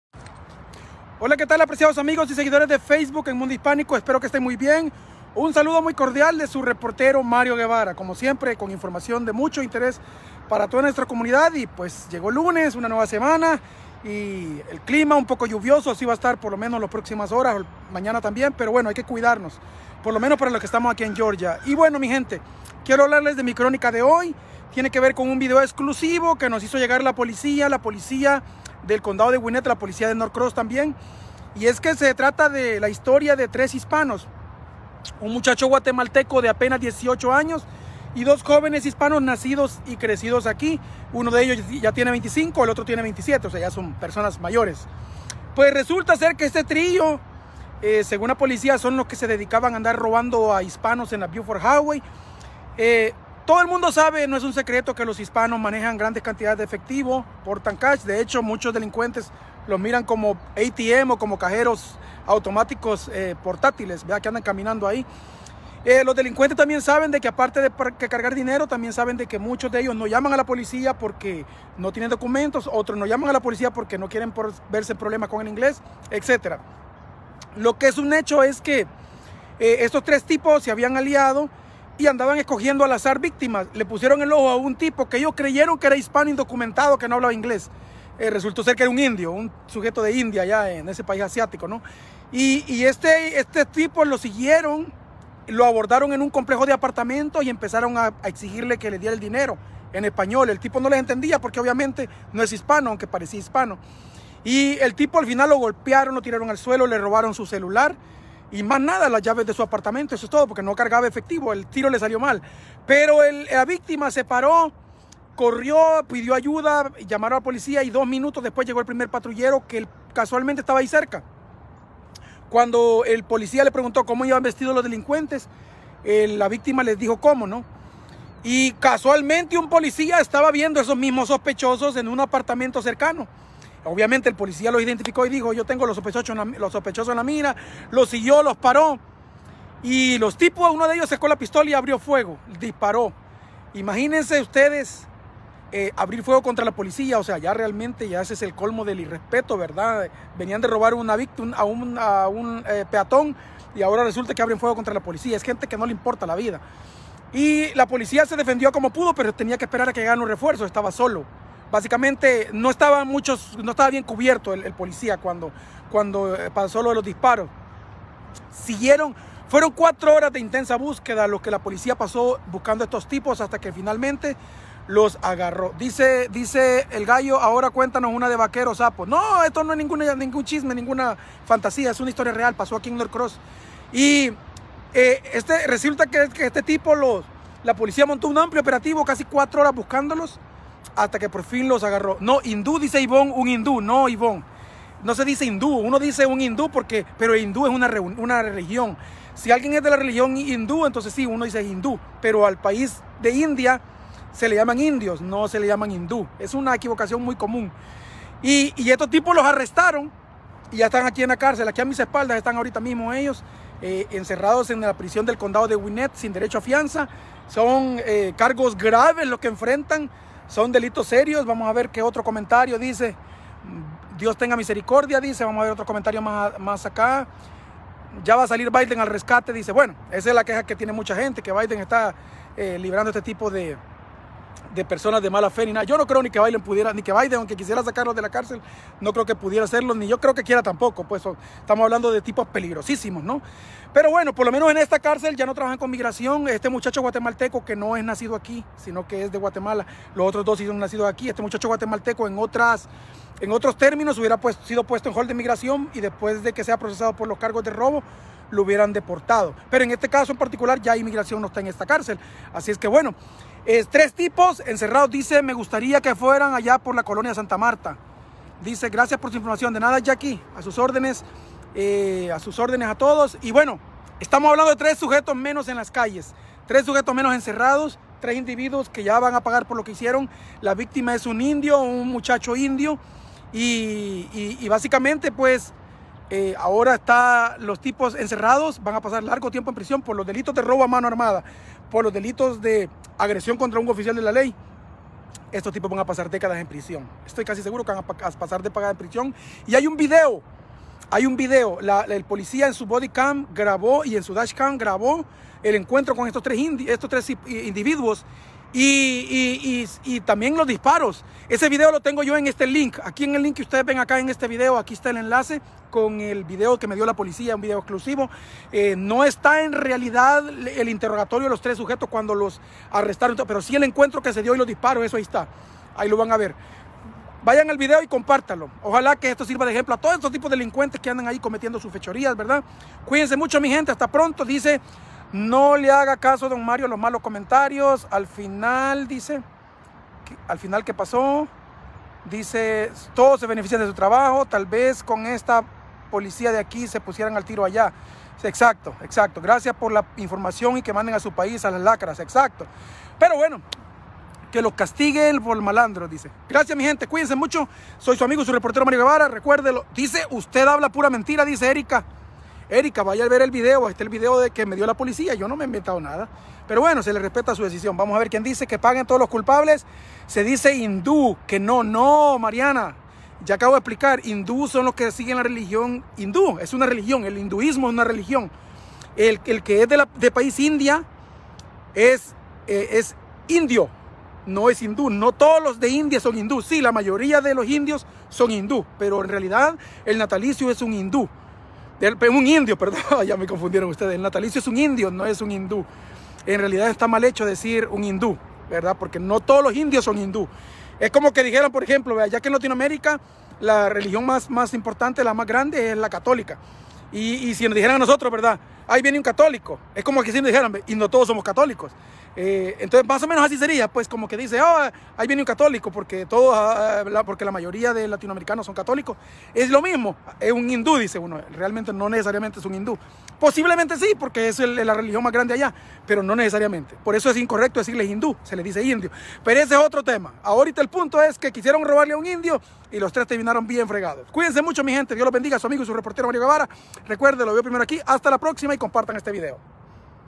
Hispanos dedicados al crimen en Norcross Georgia son arrestados | Noticias
Quién sabe en qué momento Guillermo Pérez conoció a Lesvi Rivera y a Erin Bonilla, pero lo que sí es un hecho es que esta mal llamada “amistad” le acaba de arruinar la vida.

Y es que el adolescente guatemalteco de tan solo 18 años está acusado de una serie de delitos graves que tienen que ver con robo, posesión ilegal de armas de fuego y hasta con atentar contra la integridad de agentes de la ley y el orden.

En lo que respecta a Rivera y Bonilla, a sus 25 y 27 años respectivamente, ya son considerados hombres mayores, así que saben perfectamente lo que hacen.